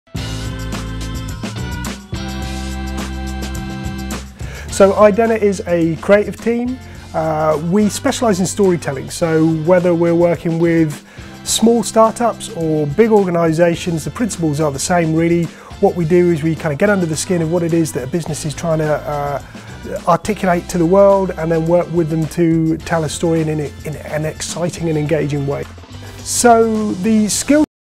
Idenna talks about the benefits of a Skills Review
James Neale, creative director for Idenna, talks about how a Skills Review gave him a useful perspective on his team's skills needs.